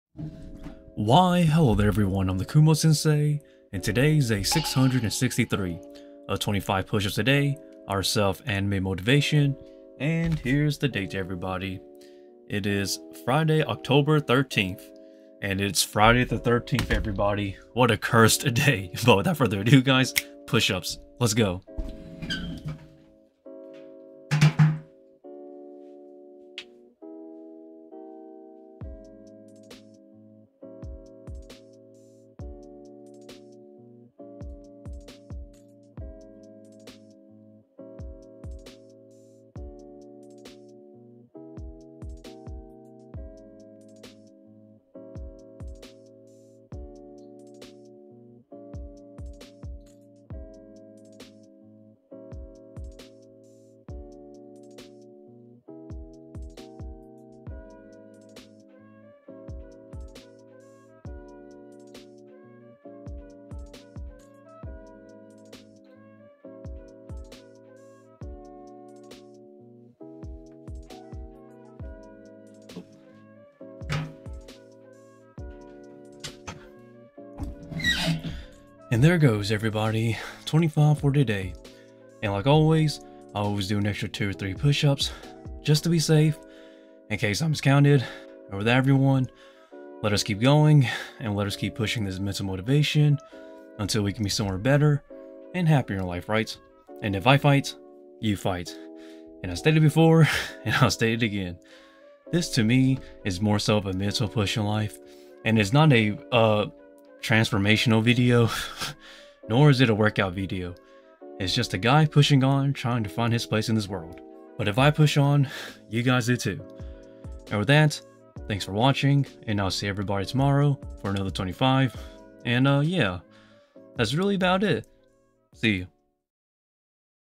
My Self Anime Motivation | 25 Push-ups Day 663
Let's talk about this self-motivation Day 663 of doing 25 push-ups. Until I can be successful and somewhere better in life.